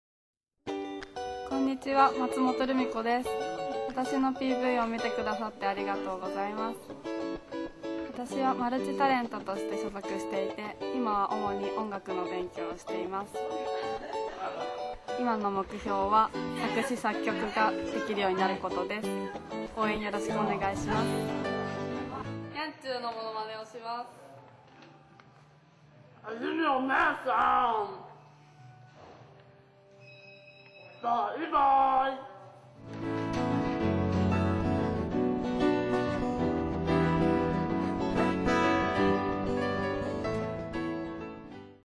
松山るみこ（タレント）PV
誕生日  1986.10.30
血液型 B型
身長 159cm
サイズ B:83 W:62 H:86
趣味：特技  歌うこと・食べること・書道(二段)・ネイルアート
ニャンチューの声・カラスの声・親指が逆に曲がる・両利き